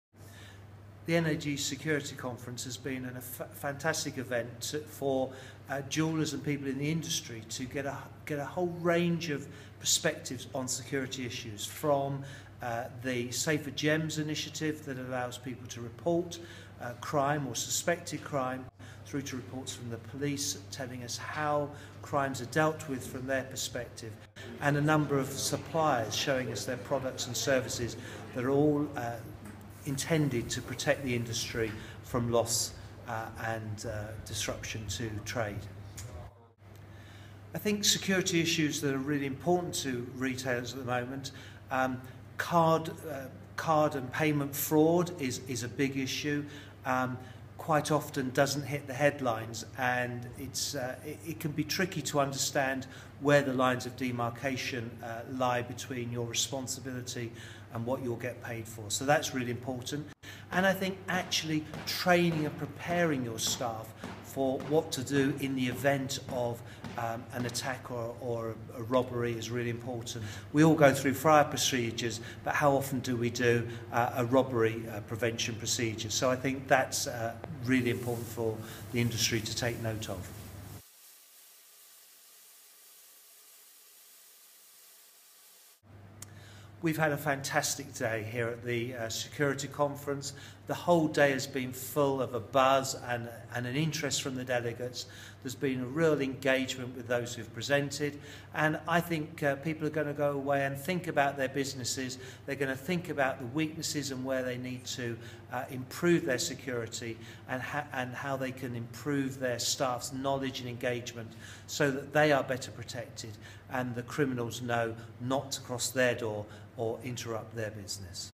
RJTV: NAG Security Conference 2014 Michael Rawlinson Interview
At the end of the NAG Security Conference 2014, RJ spoke to chief executive Michael Rawlinson to get his view on the outcome and reception of delegates following the event.